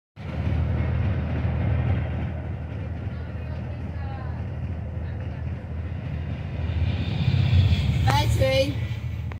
A Design Inbound Metra Train Up Close in Bartlett, Illinois. 9/30/24.
I was on my way to school that Monday morning when I saw this Inbound Metra Train coming up close so I hope you folks enjoy this Inbound Metra Train Up Close.